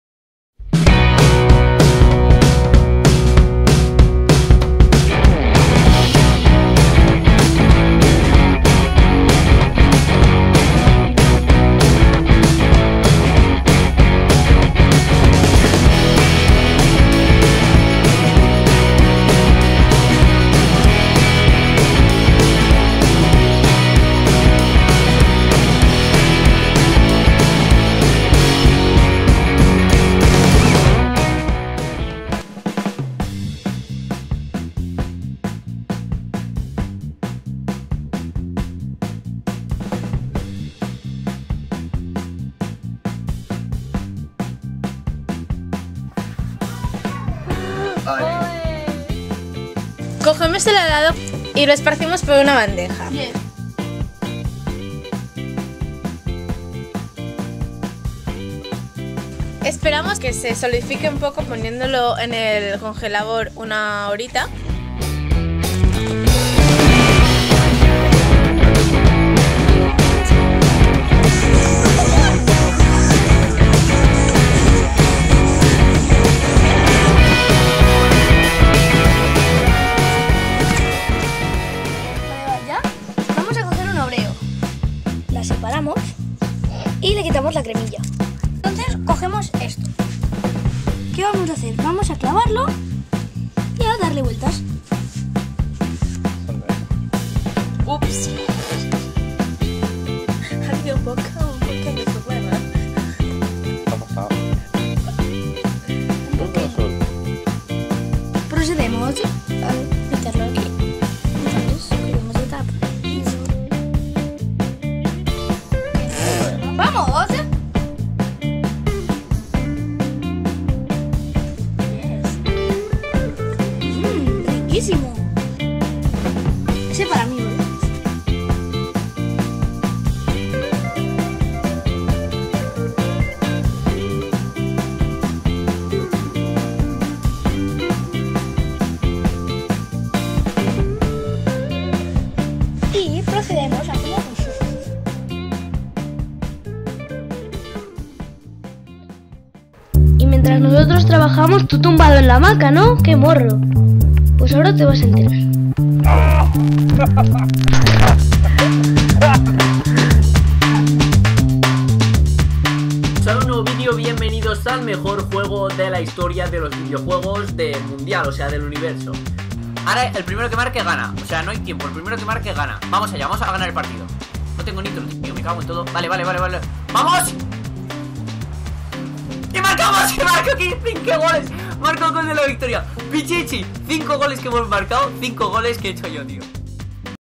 Sandwiches DE HELADO con OREO - YOCOMO
Sandwiches de Helado con OREO
Ya ves qué fácil y divertido puede ser hacer tus propios sanwiches de helado con galletas oreo.